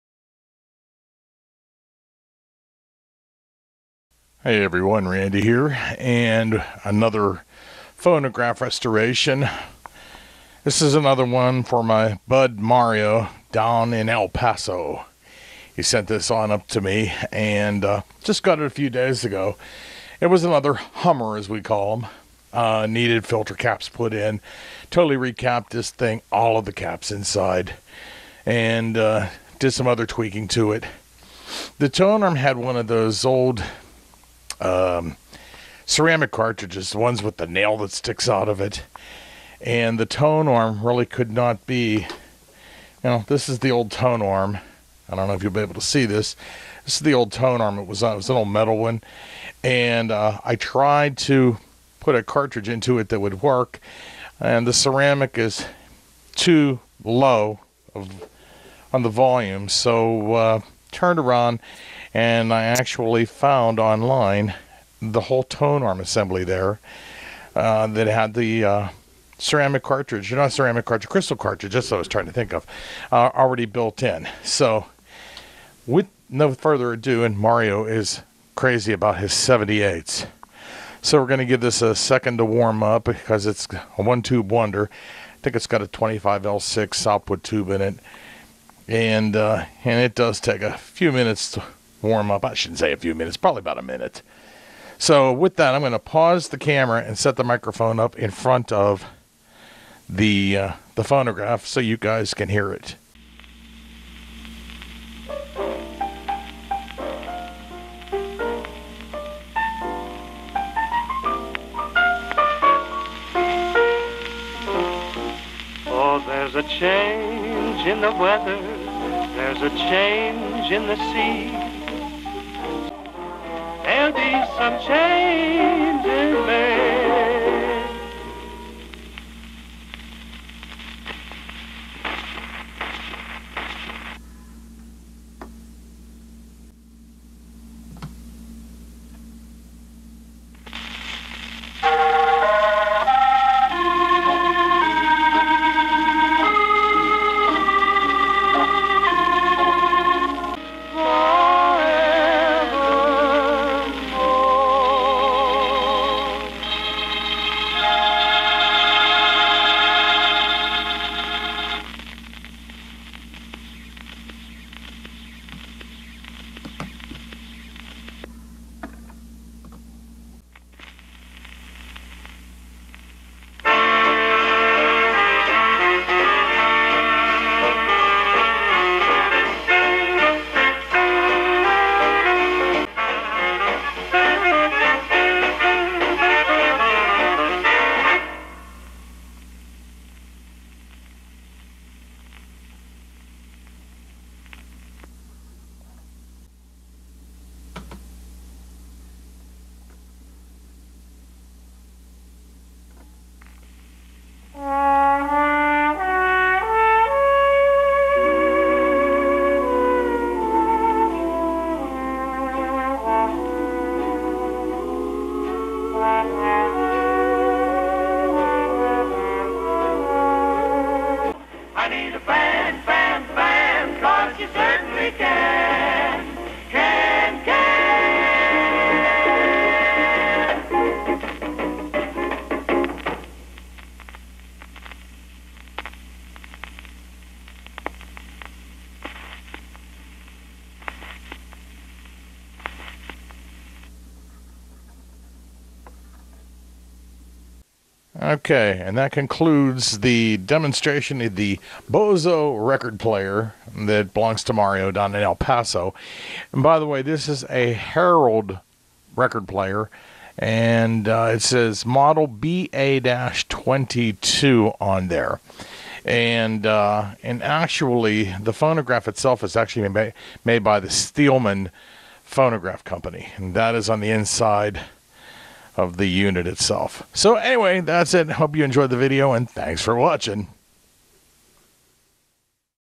Bozo Record Player from the 1950's
A Bozo kid's phonograph from the 1950's. Just recapped it, replaced the tonearm & cartridge, and now, shot this video to show it working. No, it's not high fidelity. It wasn't meant to be. I hope you enjoy the video.